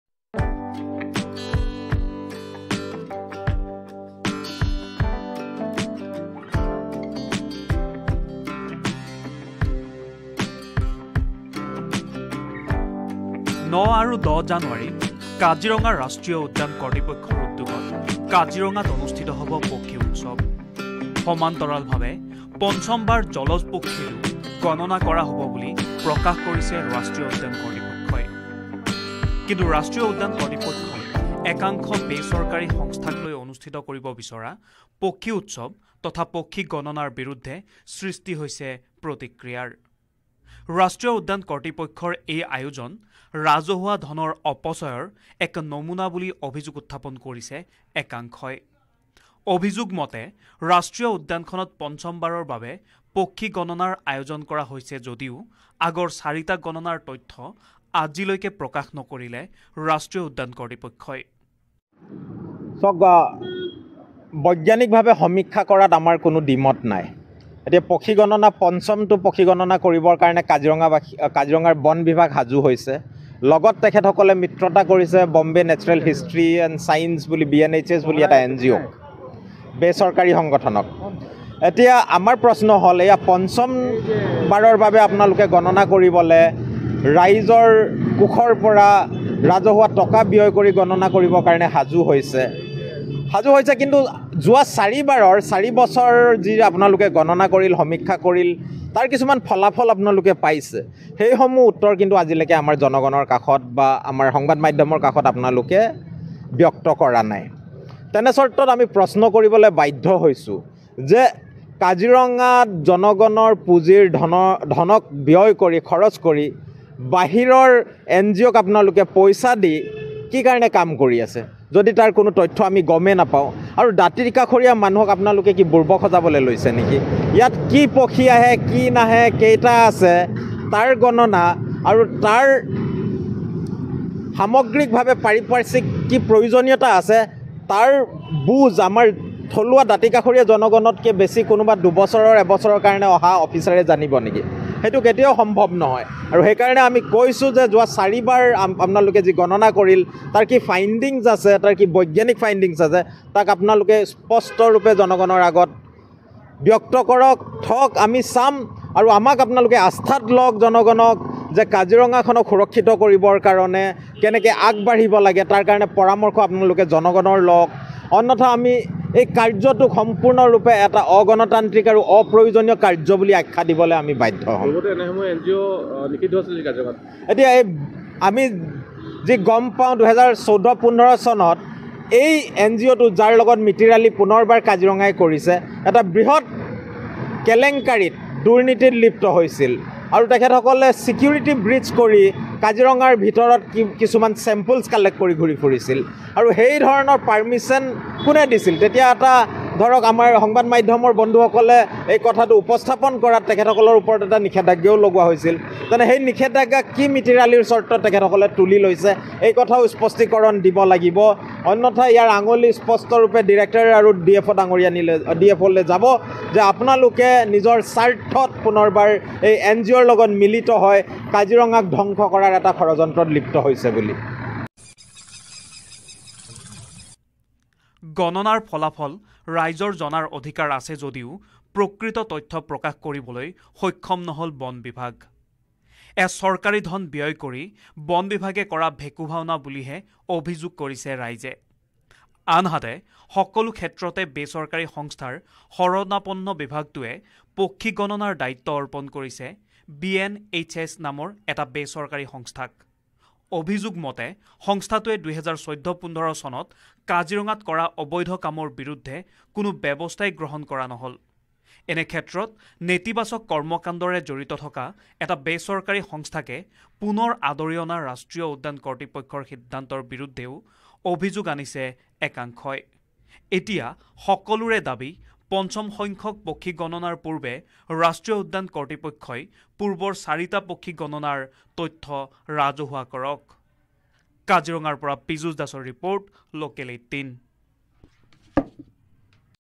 Kaziranga Bird : ৯ আৰু ১০ জানুৱাৰীত কাজিৰঙাত অনুষ্ঠিত হ’ব পক্ষী উৎসৱ ৷
Kaziranga Bird | ৯ আৰু ১০ জানুৱাৰীত কাজিৰঙা ৰাষ্ট্ৰীয় উদ্যান কৰ্তৃপক্ষৰ উদ্যোগত কাজিৰঙাত অনুষ্ঠিত হ’ব পক্ষী উৎসৱ ৷ সামান্তৰালভাৱে পঞ্চম বাৰ জলজ পক্ষীৰো গণনা কৰা হ’ব বুলি প্ৰকাশ কৰিছে ৰাষ্ট্ৰীয় উদ্যান কৰ্তৃপক্ষই৷ কিন্তু ৰাষ্ট্ৰীয় উদ্যান কৰ্তৃপক্ষই একাংশ বেচৰকাৰী সংস্থাকলৈ অনুষ্ঠিত কৰিব বিচৰা পক্ষী উৎসৱ তথা পক্ষী গণনাৰ বিৰুদ্ধে সৃষ্টি হৈছে প্ৰতিক্ৰিয়াৰ।